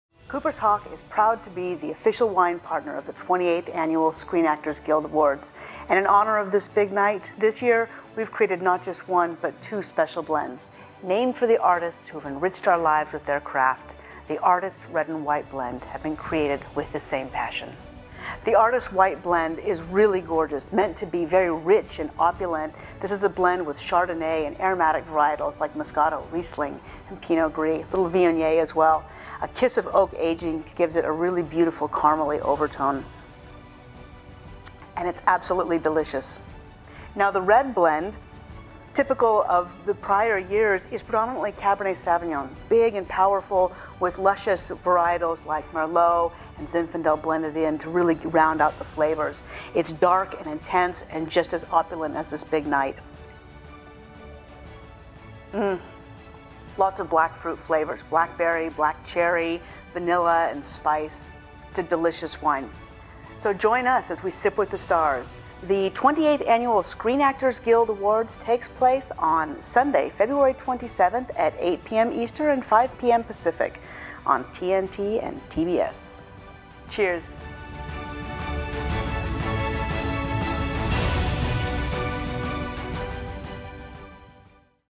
SAG Awards Artist's Red & White Blends - Cooper's Hawk March 2022 Wine of the Month
Not a Member?

Cooper’s Hawk is the official wine partner for the Screen Actors Guild Awards, and this year, we’re introducing Hollywood’s newest “it couple”: Artist’s Red & White Blends! This Wine of the Month is exclusively available to Wine Club Members and Screen Actors Guild Award nominees.

Artist’s Red Blend is luxurious and unique, while our new Artist’s Whie Blend is rich and opulent. Named for the artists that have enriched our lives with their craft, the Artist’s Red & White Blend have been created with the same passion.

Join the 1 bottle per month Club for $21.99 by March 22 to take home our Artist’s Red or White, or choose the 2 or 3-bottle to try both blends. Enjoy your bottles in our dining room, or take them home to enjoy during the Screen Actors Guild Awards on Sunday, February 27 at 9pm EST on TBS or TNT. Cheers to you and all of the nominees!

Retail items and wines available for purchase (can be shipped)